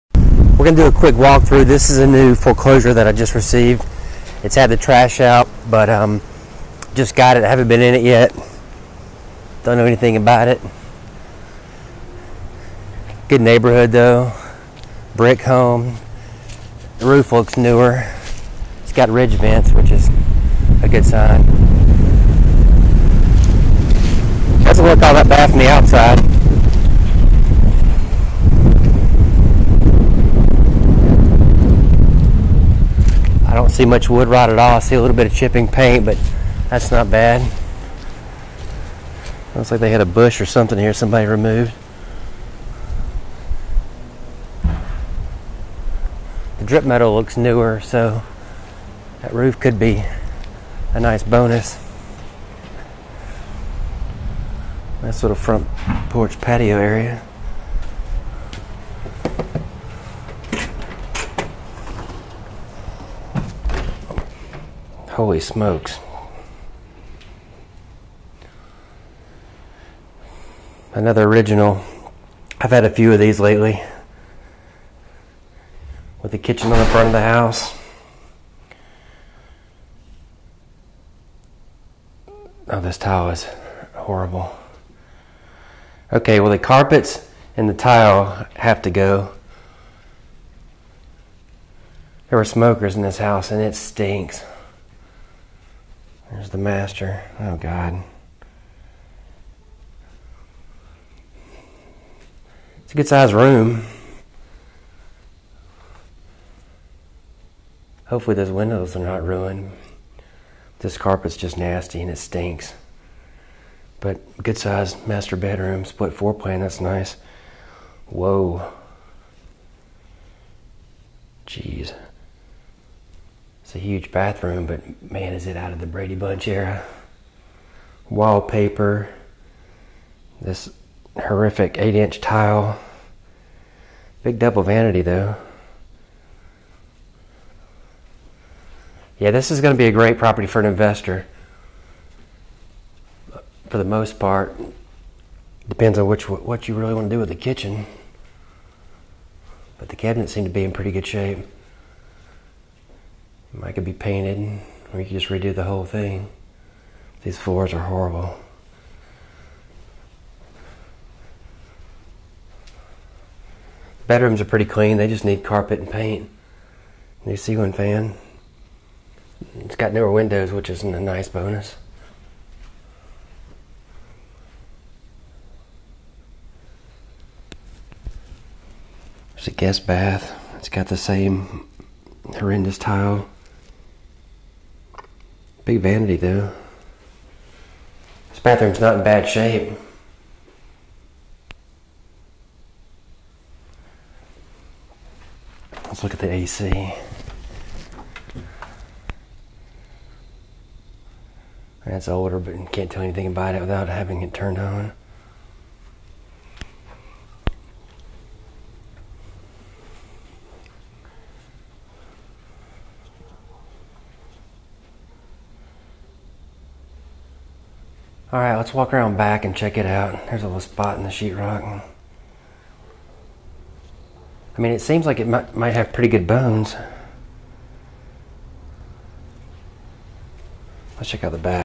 New Foreclosure - 10851 Tara Dawn Circle Pensacola 32533
New Foreclosure and this will be a great opportunity for an investor. Home needs cosmetic repairs. The previous owner was a smoker and it's obvious.